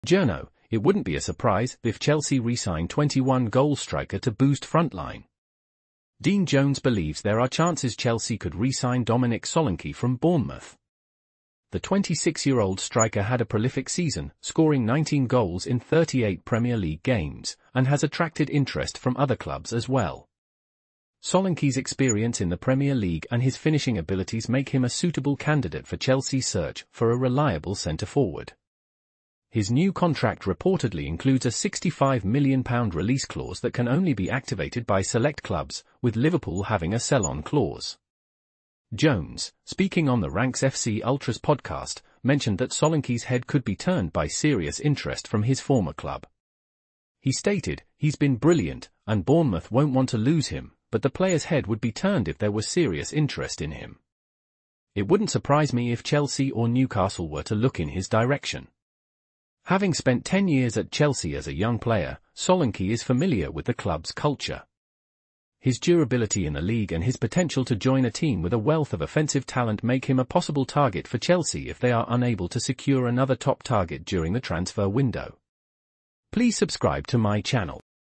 Journo: “It wouldn’t be a surprise” if Chelsea re-sign 21-goal striker to boost frontline
Dean Jones believes there are chances Chelsea could re-sign Dominic Solanke from Bournemouth. The 26-year-old striker had a prolific season, scoring 19 goals in 38 Premier League games, and has attracted interest from other clubs as well.
Solanke's experience in the Premier League and his finishing abilities make him a suitable candidate for Chelsea's search for a reliable centre-forward. His new contract reportedly includes a £65 million release clause that can only be activated by select clubs, with Liverpool having a sell-on clause.
Jones, speaking on the Ranks FC Ultras Podcast, mentioned that Solanke's head could be turned by serious interest from his former club. He stated, "He’s been brilliant, and Bournemouth won’t want to lose him, but the player’s head would be turned if there was serious interest in him. It wouldn’t surprise me if Chelsea or Newcastle were to look in his direction."
Having spent ten years at Chelsea as a young player, Solanke is familiar with the club's culture. His durability in the league and his potential to join a team with a wealth of offensive talent make him a possible target for Chelsea if they are unable to secure another top target during the transfer window.